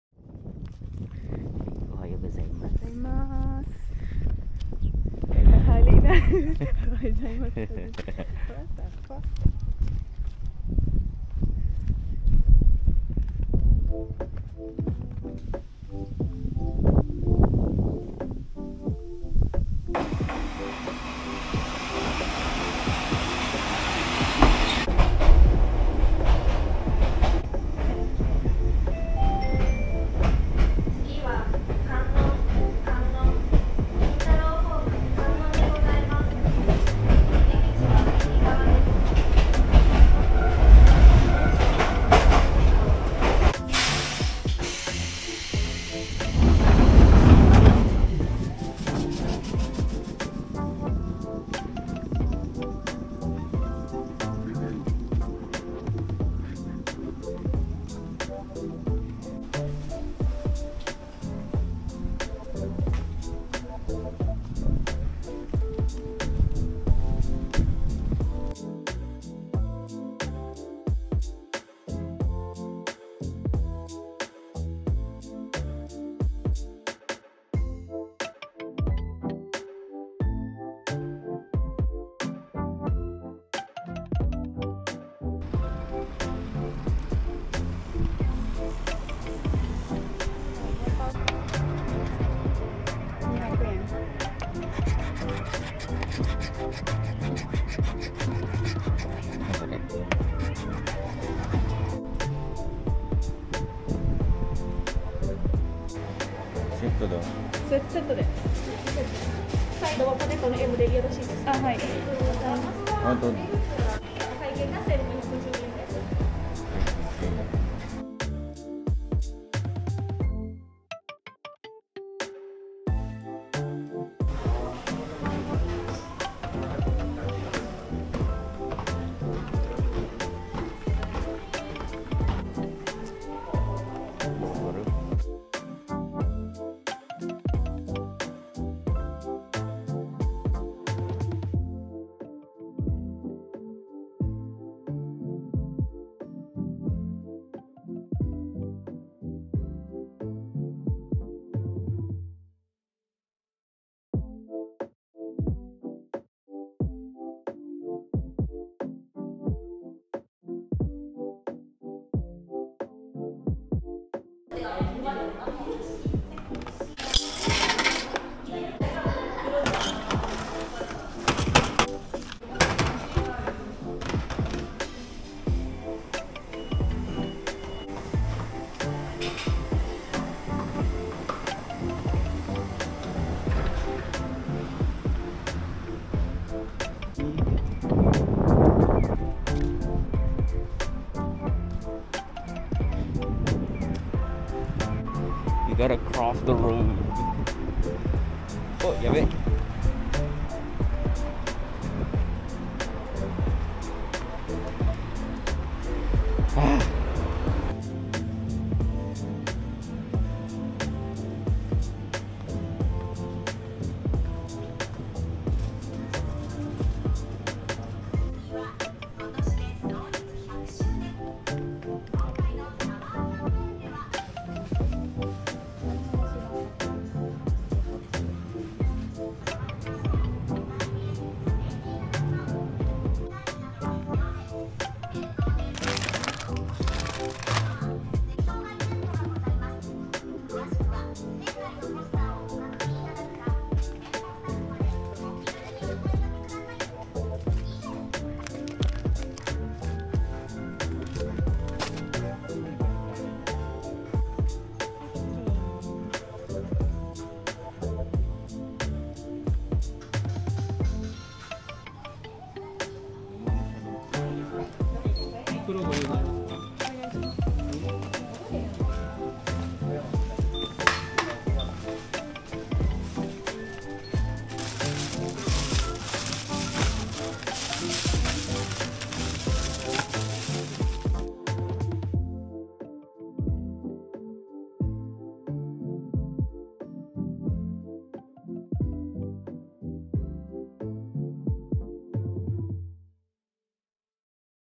Mini POV vlog BACK TO JAPAN i accidentally pressed the go back button | zkael★
I'm just sad we could't film our flight, some interesting thing happened back there, 2 Japanese speaker passengers we're fighting and shouting, It could have been a massive content. lmao i'm just kidding. But yea this is all for now, we just came back and still quite busy going here and there for paper work and stuff it's complicated, JAPAN IS VERY COMPLICATED tm.